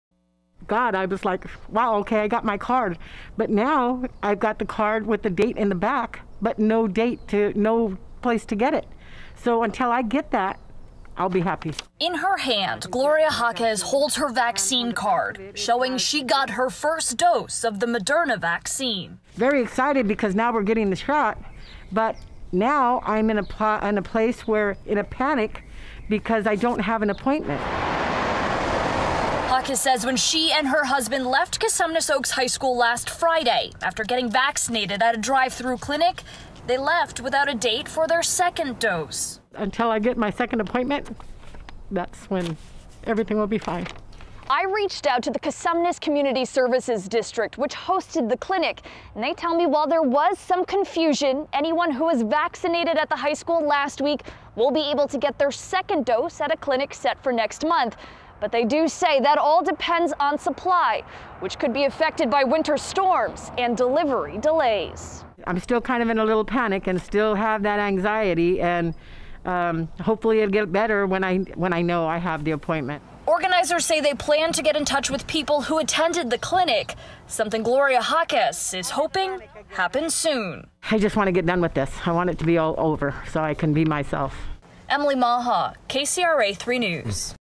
Vaccine delays leave some questioning second-dose availability
The winter storm that has crippled parts of the country is delaying deliveries of the COVID-19 vaccine.  The California Department of Public Health said the vaccines shipped to the state have been delayed.  Public health officials in several Northern California counties said they’re tracking supply to make sure they have enough second doses for people.  Both Pfizer and Moderna require two doses of the COVID-19 vaccine.  See more in the video above.